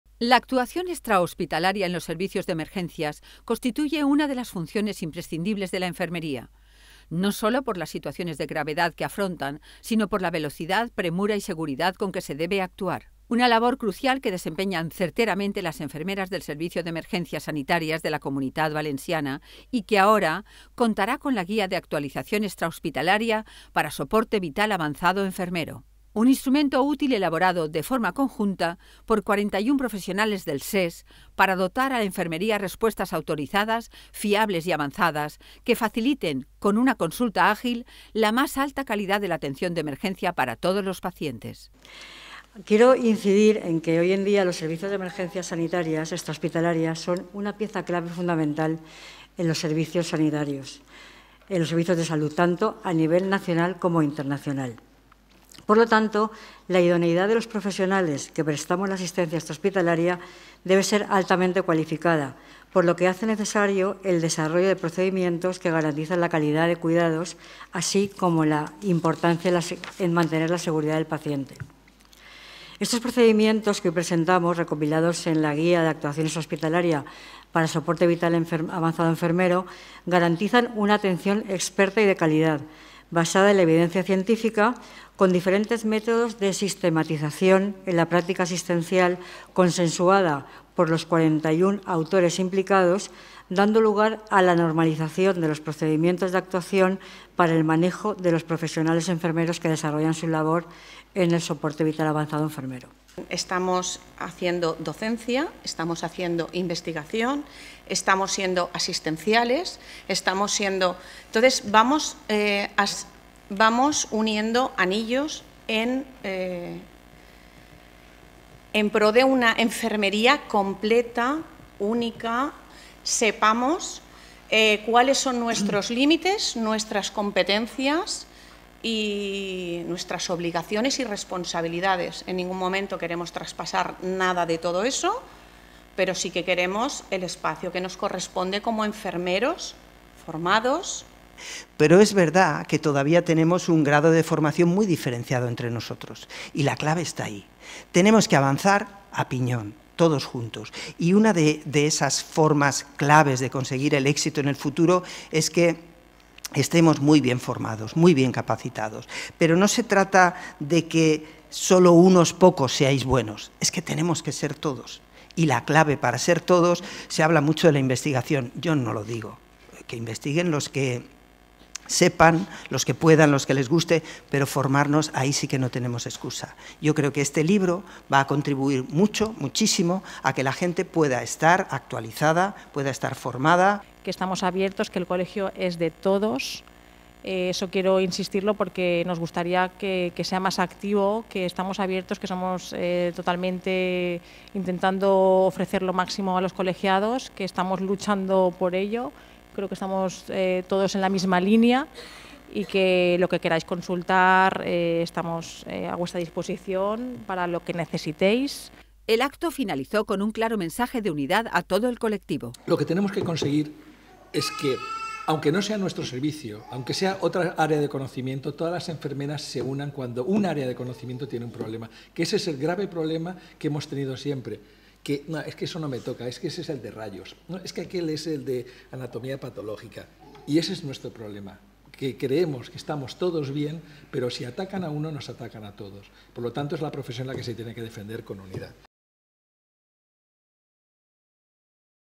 Presentación de la “Guía de Actualización Extrahospitalaria para SVAE”| Cecova TV
El pasado 21 de febrero Ediciones CECOVA presentó su nueva publicación:
“Guía de Actualización Extrahospitalaria para Soporte Vital Avanzado Enfermero” elaborada por profesionales del SES.